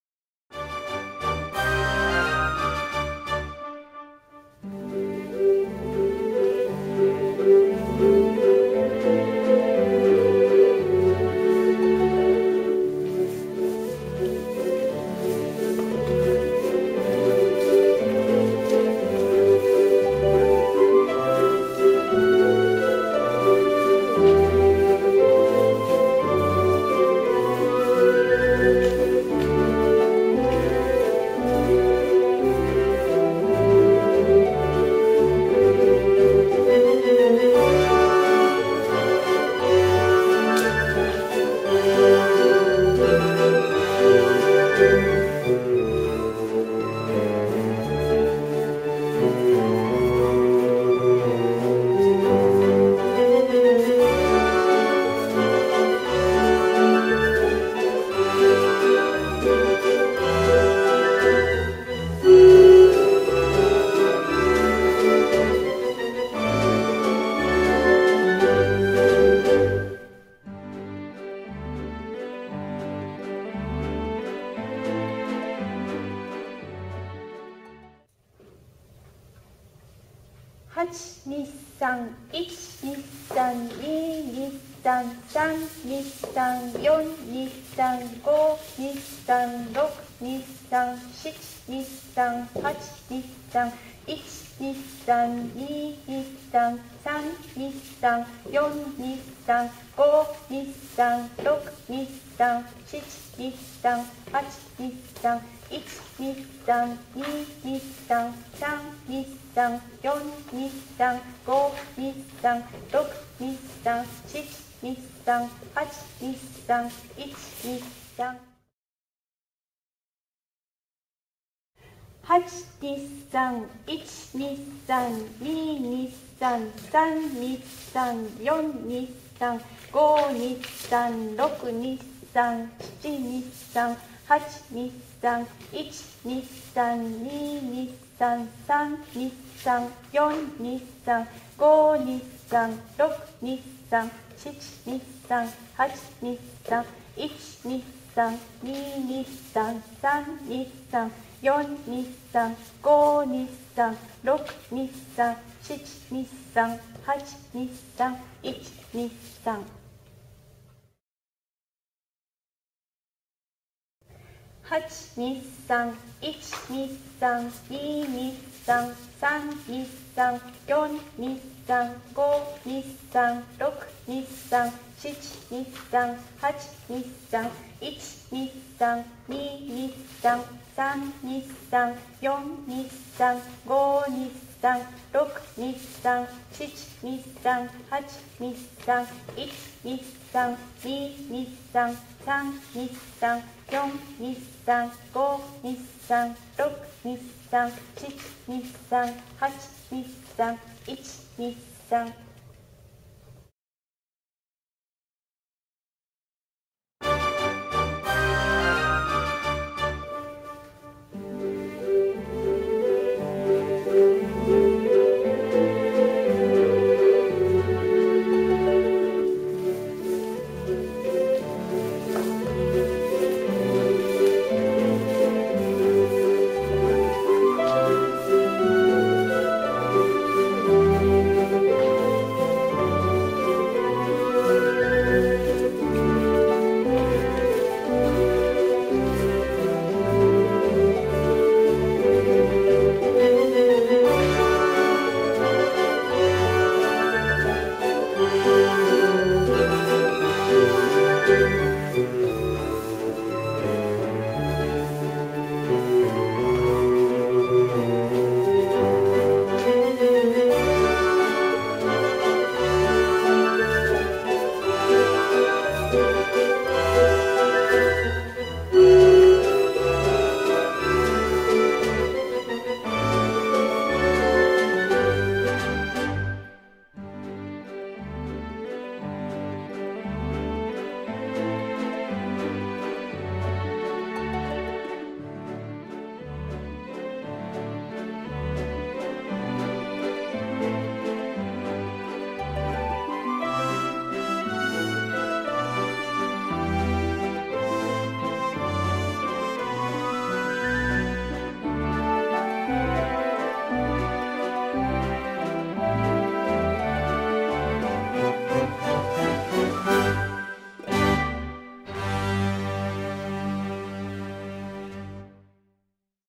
社交ダンス ウインナワルツ ベーシックステップ ヤングサークルほほえみ
「ヤングダンスサークルほほえみ」 の講習会で使用する、ウインナワルツのベーシックステップです。
ご紹介する主なステップは
ナチュラルターン、リバースターン、フォワードチェンジ、バックワードチェンジ、ナチュラルフレッカール、コントラチェック